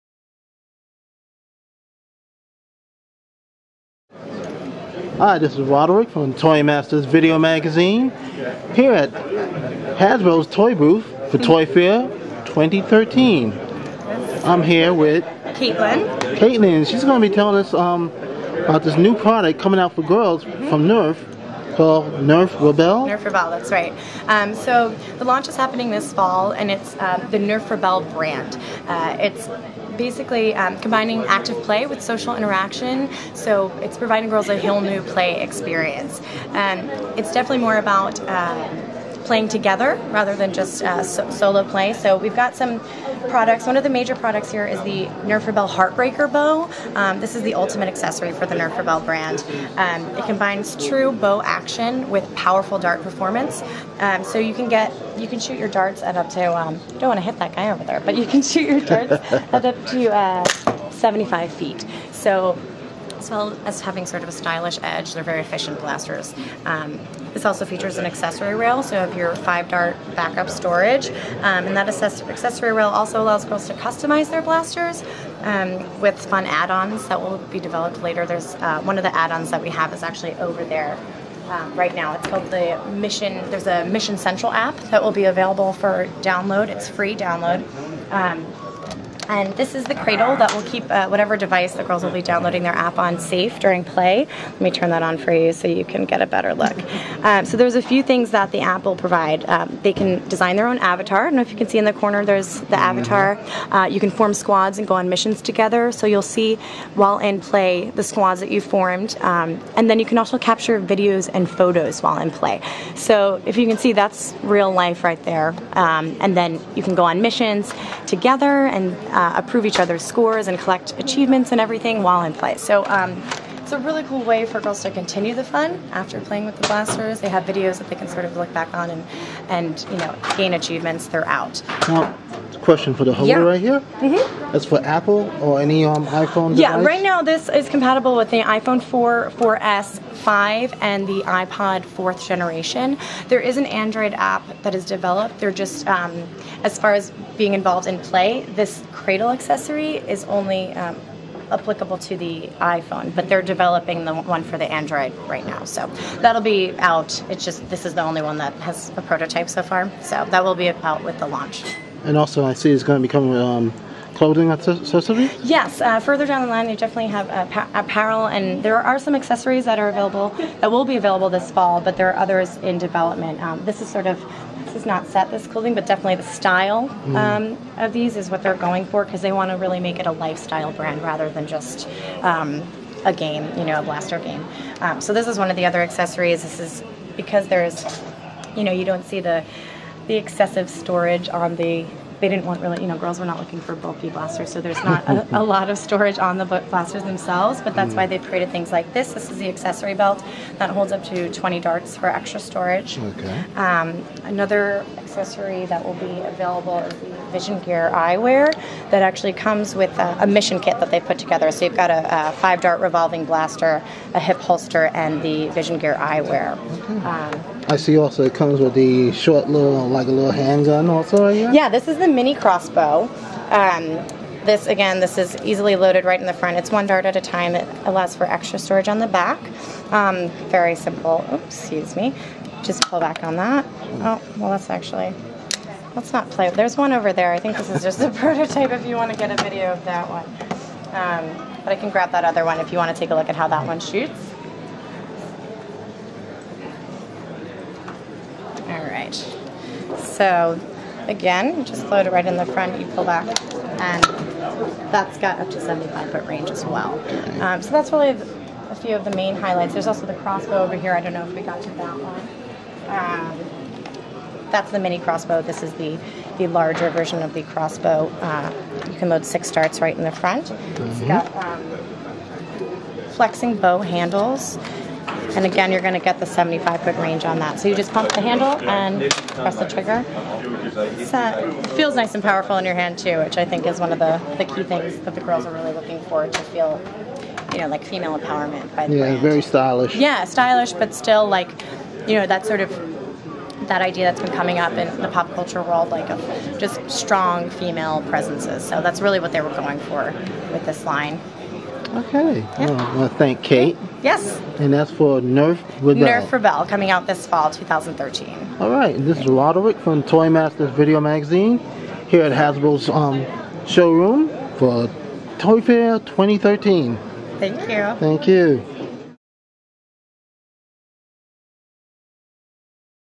Hasbro Nerf Rebelle at ToyFair 2013.
New Nerf Toy Line for girls. From Hasbro at ToyFair 2013.